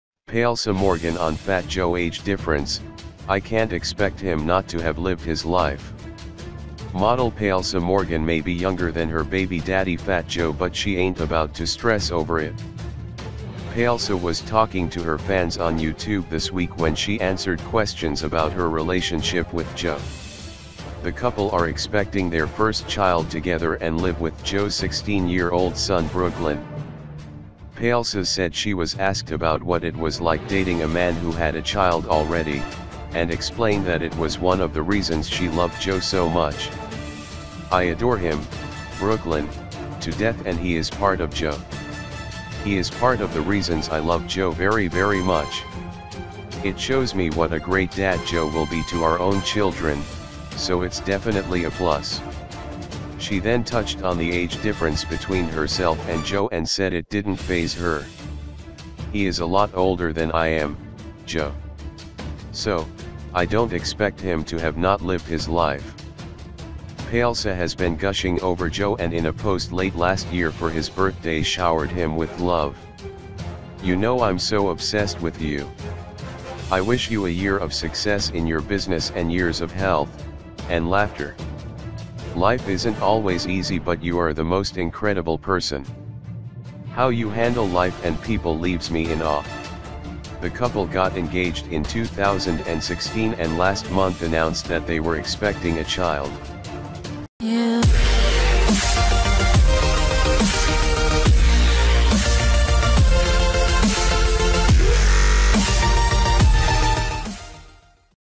Palesa Morgan on Phat Joe age difference: I can’t expect him not to have lived his life
Model Palesa Morgan may be younger than her baby daddy Phat Joe but she ain't about to stress over it.
Palesa was talking to her fans on YouTube this week when she answered questions about her relationship with Joe.
The couple are expecting their first child together and live with Joe's 16-year-old son Brooklyn.
Palesa said she was asked about what it was like dating a man who had a child already, and explained that it was one of the reasons she loved Joe so much.
"I adore him (Brooklyn) to death and he is part of Joe. He is part of the reasons I love Joe very very much. It shows me what a great dad Joe will be to our own children, so it's definitely a plus".
She then touched on the age difference between herself and Joe and said it didn't phase her.
"He is a lot older than I am, Joe. So, I don't expect him to have not lived his life."